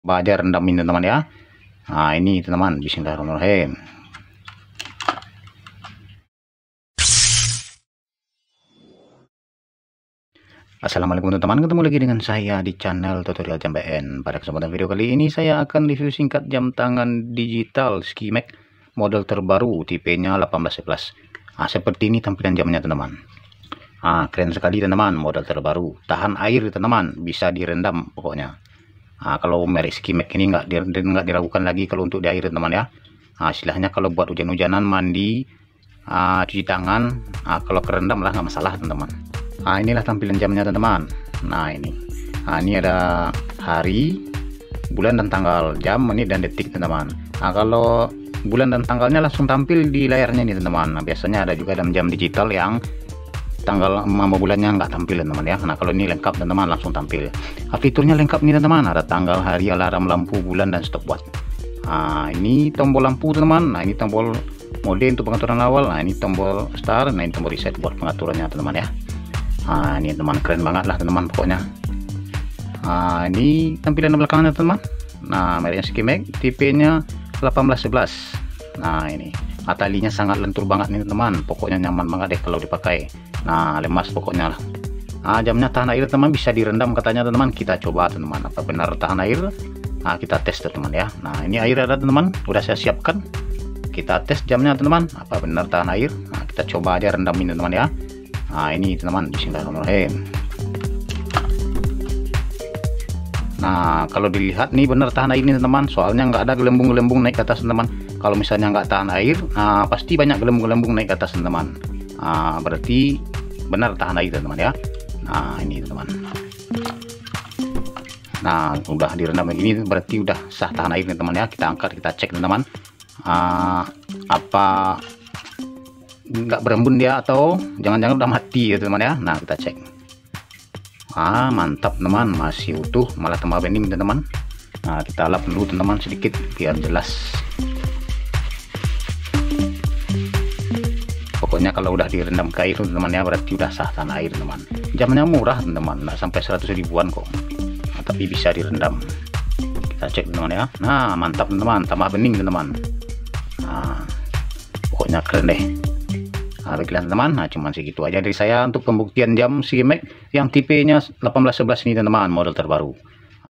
Review jam tangan SKYMAX 1811 terbaru || tahan air
Assalamualaikum kawan kawan semua..
pada kesempatan video kali ini saya akan review jam tangan digital SKYMAX 1811 model terbaru dan tahan air, nah mari tonton video saya berikut ini sampai selesai